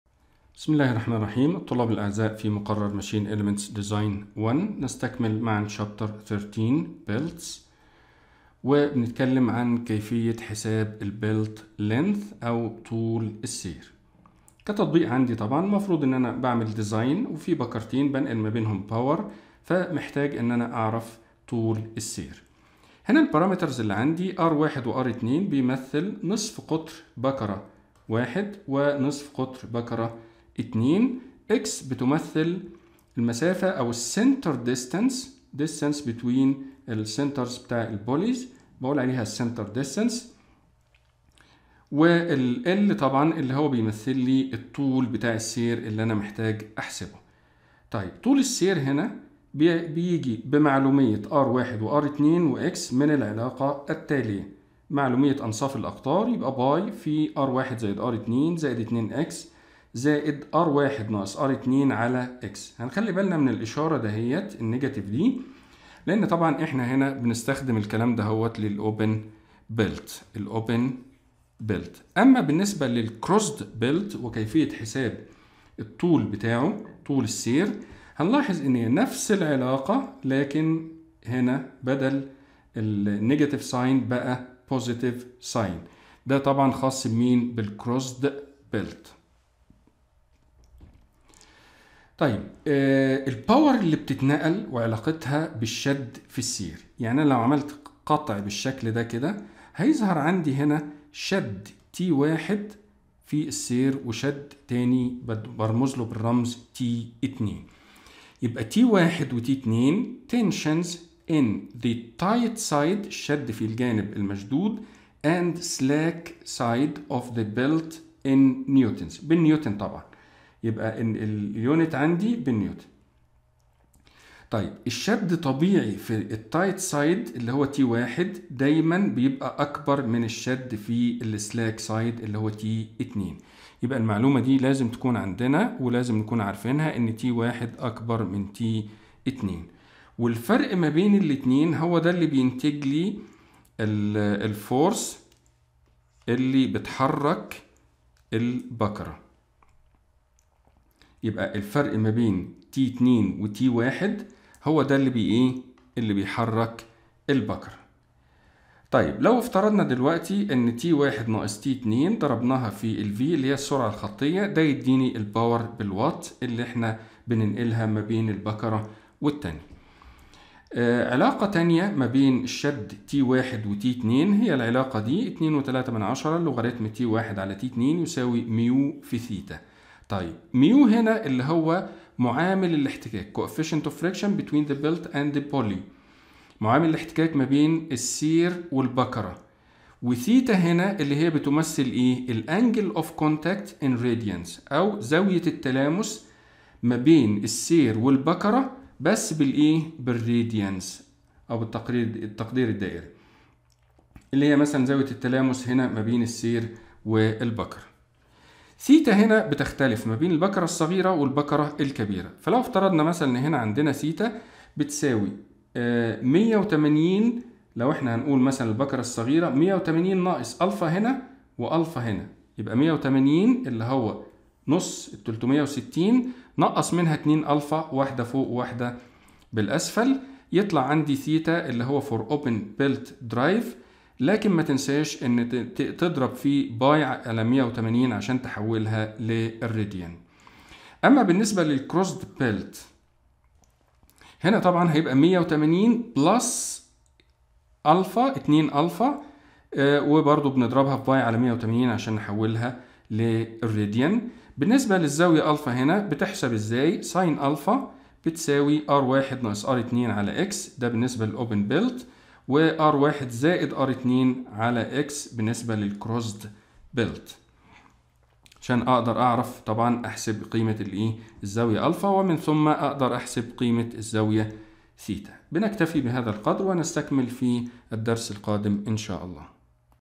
Machine Elements Design - 1 | 13-5 | Belt Length, and Tension
المقررات المفتوحة - تصميم أجزاء الماكينات - Machine Elements Design - 1
الوحدة 13 : Belts
الدرس 5 : Belt Length, and Tension
- Power Transmitted by Belt.
- Ratio of Driving Tensions for Flat Belt Drive.